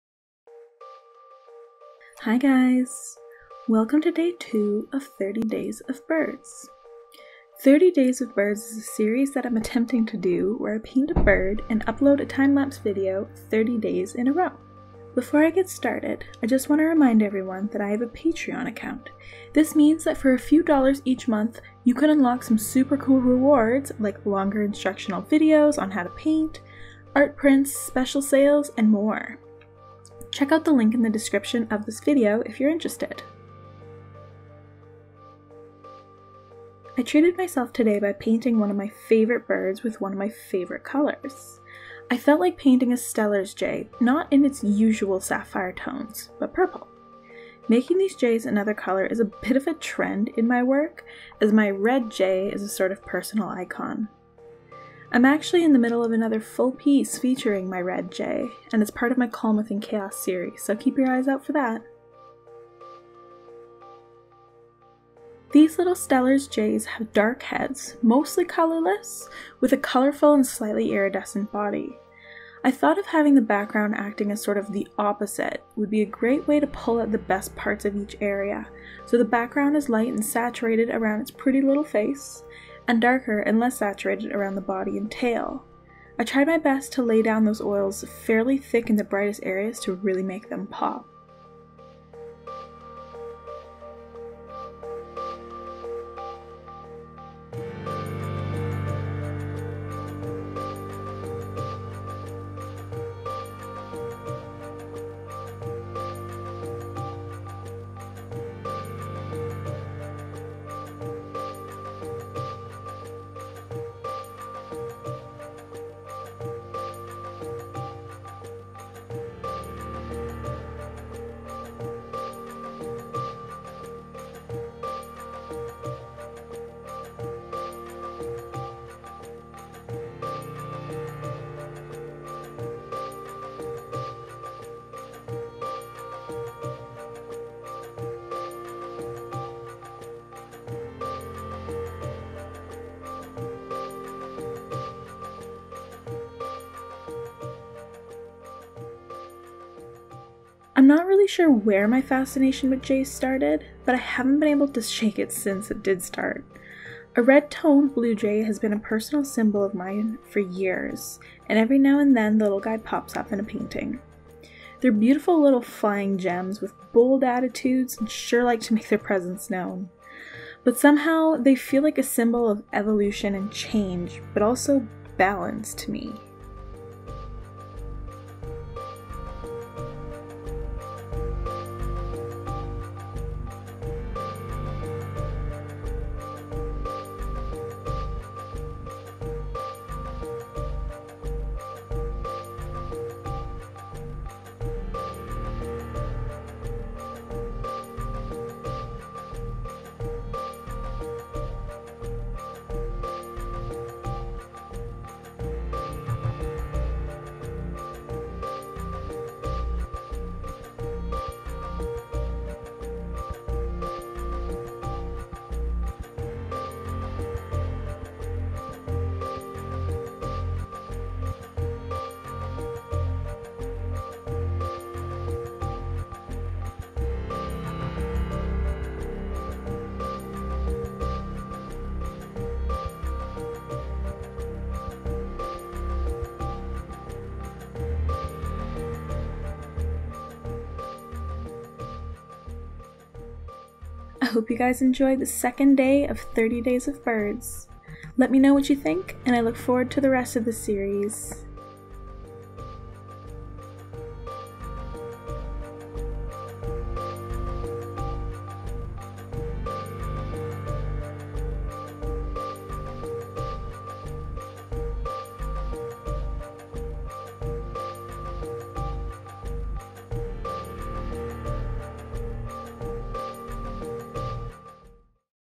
Day 2 - 30 Days of Birds - Purple Steller's Jay
So I have decided to give myself a hard challenge - 30 days of birds!
My plan is to have 30 uploads in a row of timelapse paintings of birds. Lets see if I can do this!

Most of this series will be painted with oil paints on paper.